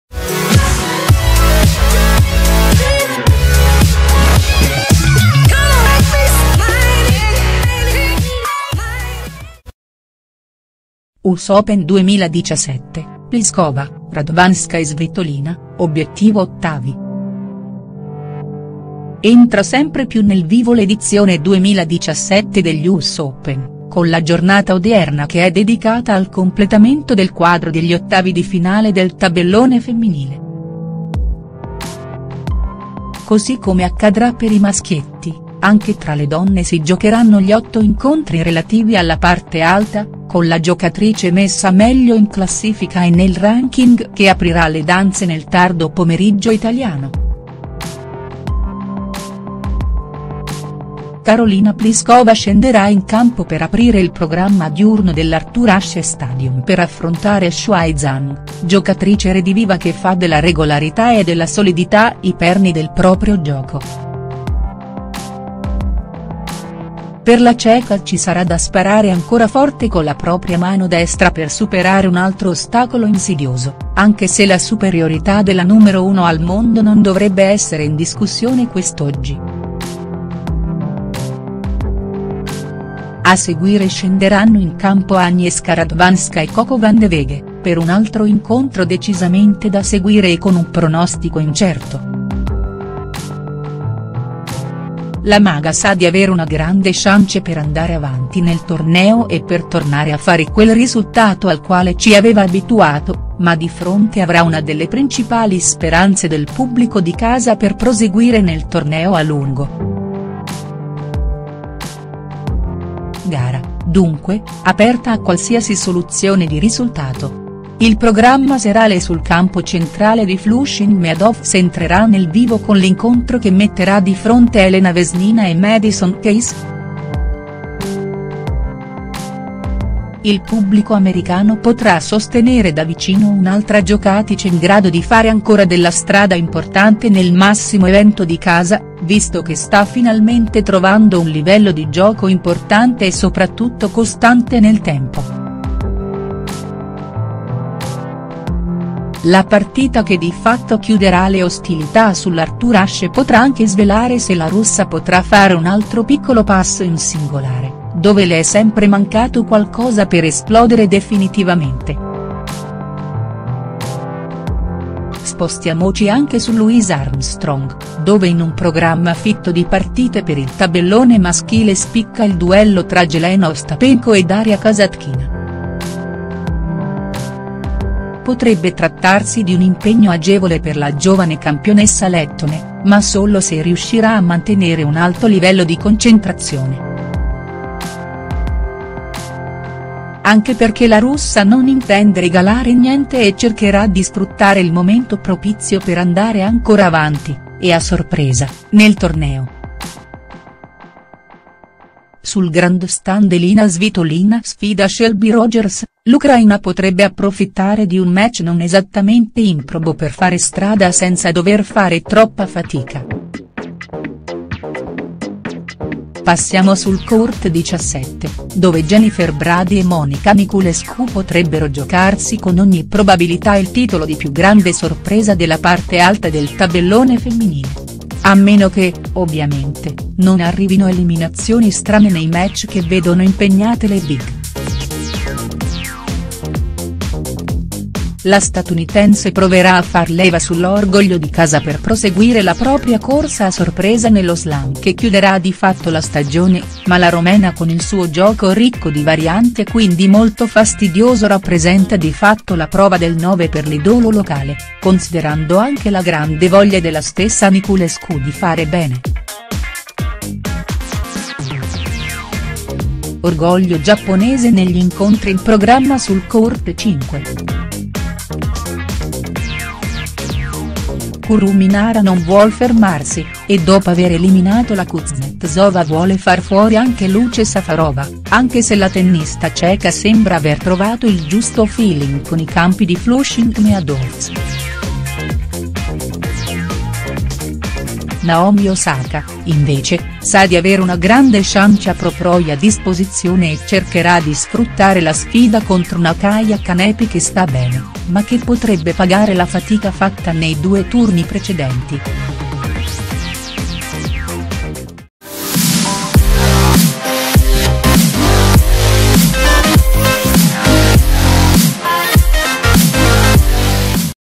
Us Open 2017 - Pliskova, Radwanska e Svitolina: obiettivo ottavi.HD
La numero 1 al mondo sfida la Zhang, ostacolo Vandeweghe per Aga, mentre l'ucraina ha un match morbido contro la Rogers. Da seguire Vesnina-Keys.